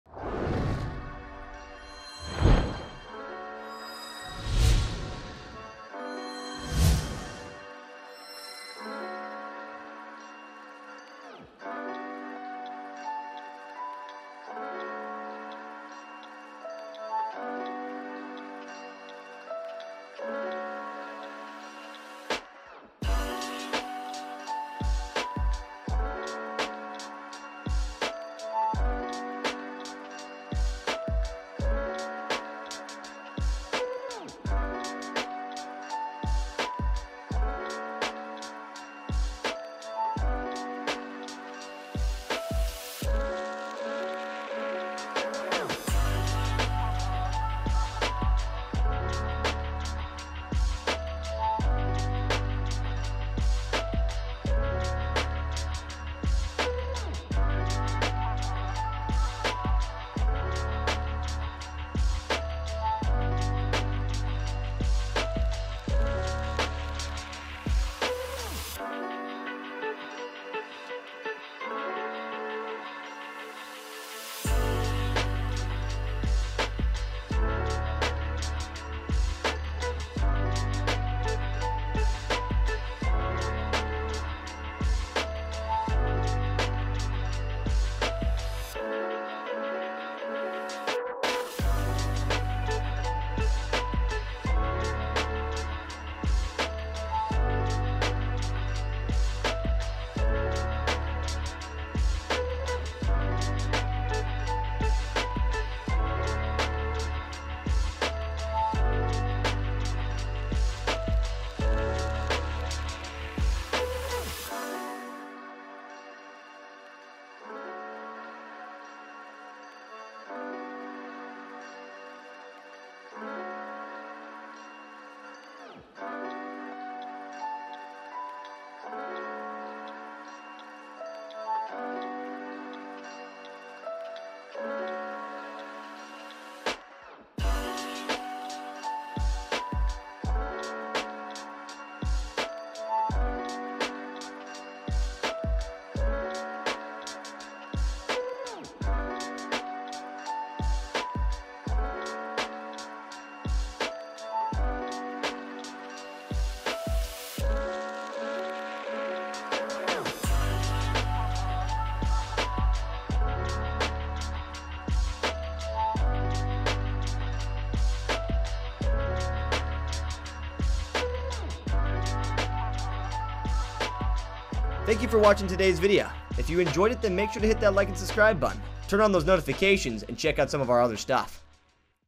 Comparison: Most Intelligent Marvel Characters
In this comparison video, we will show you the most intelligent Marvel characters. Is it an MCU favorite like the Scarlet Witch, Spider-Man or Doctor Strange? How much smarter is Bruce Banner than the Hulk? What is Tony Stark's IQ? How intelligent is Thanos? Watch this video to find out who the most intelligent Marvel character is!